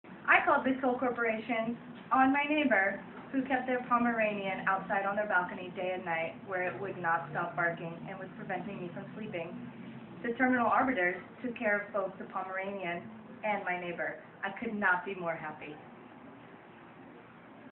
THE KILL CORPORATION - "Kate" Testimonial
A heartfelt testimonial from a client. Do you want a better life? and submit a grievance of your own.